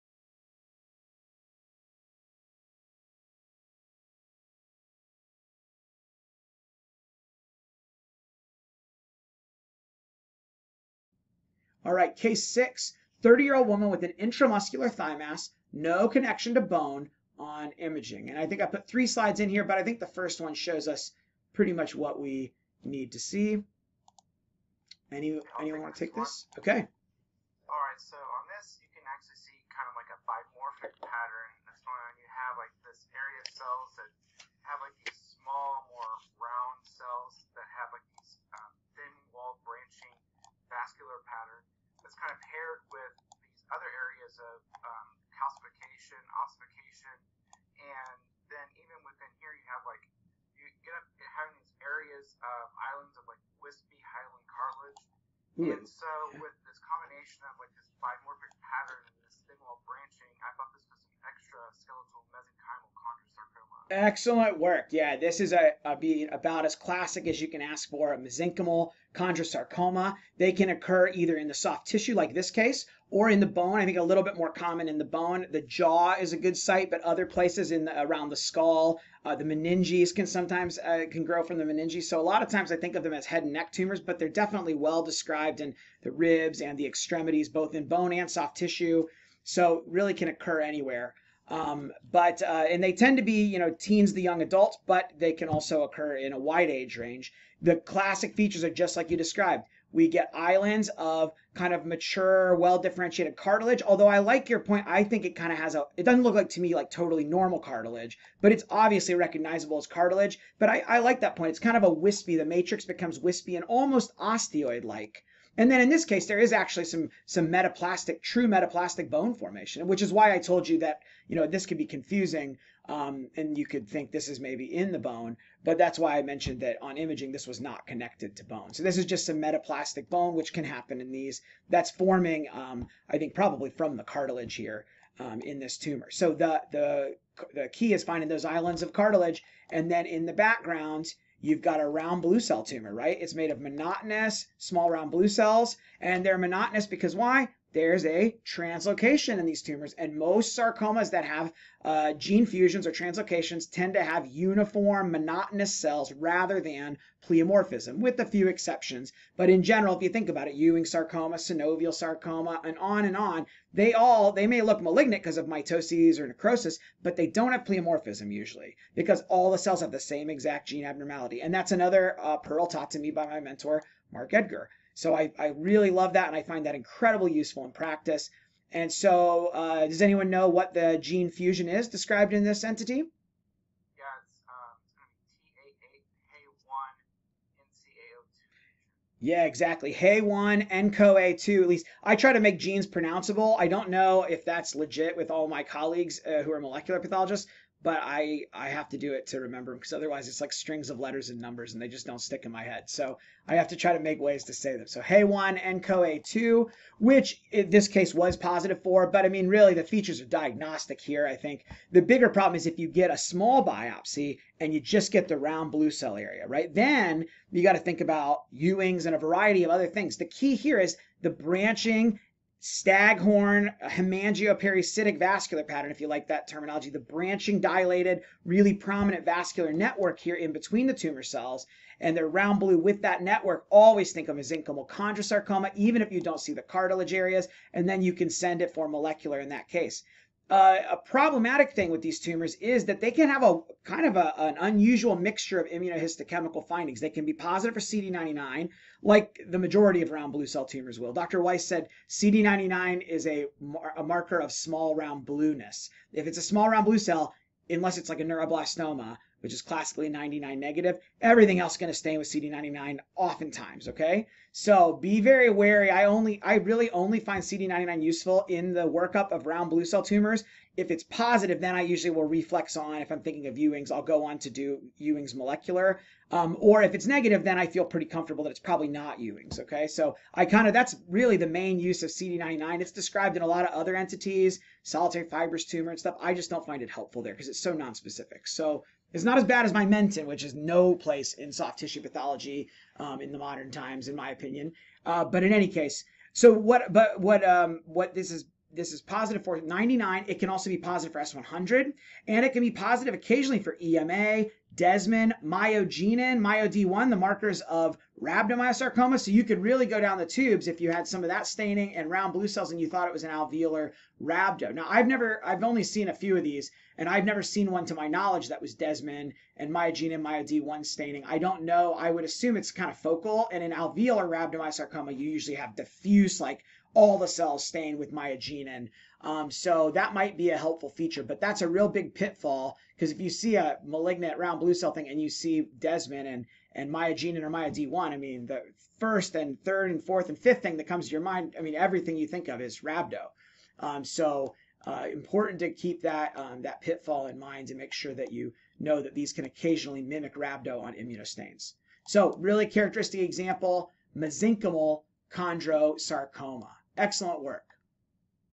Mesenchymal Chondrosarcoma - round blue cell sarcoma with cartilage & staghorn vessels (UNC case 6)
This is an excerpt from my virtual slide session for the University of North Carolina (UNC) pathology residents on 4.8.2021. Full video of the entire session is available for free WSI Digital slide for this case